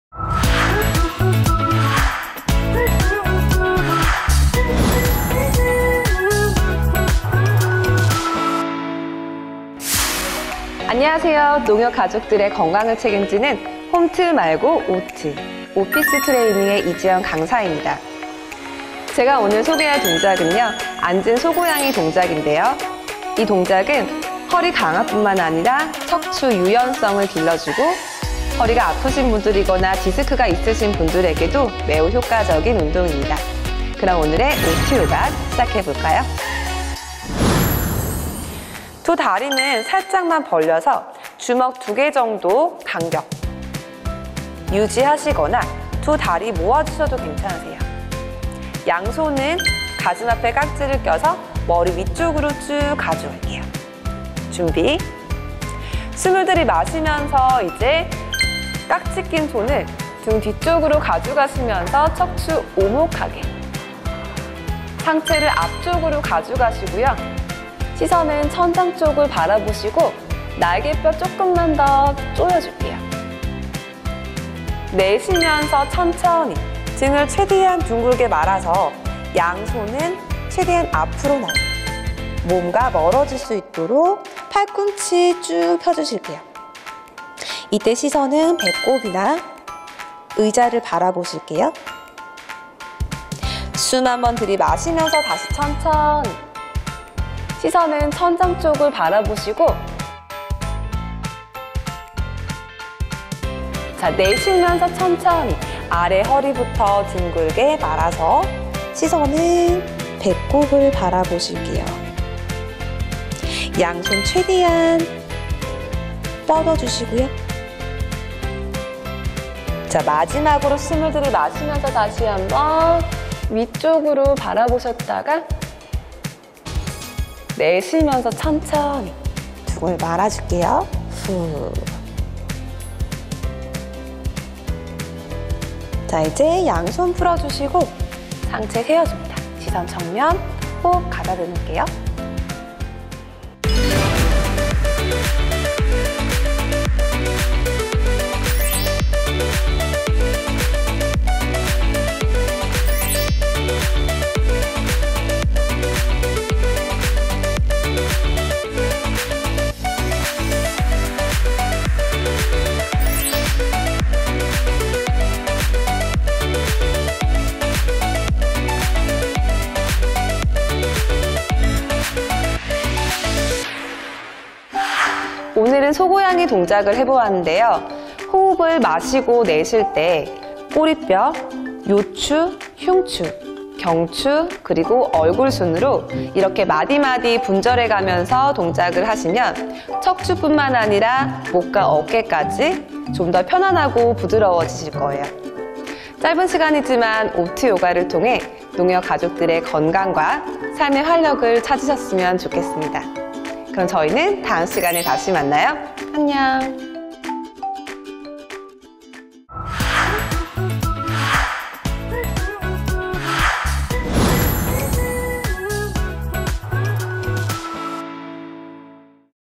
농협 오피스트레이닝 6회 - 앉은 소고양이 자세
농협과 함께하는
튼튼한 허리를 만들어주는
허리디스크와 허리통증에 좋은 앉은소고양이자세 🐂🐈운동법 배워봐요~!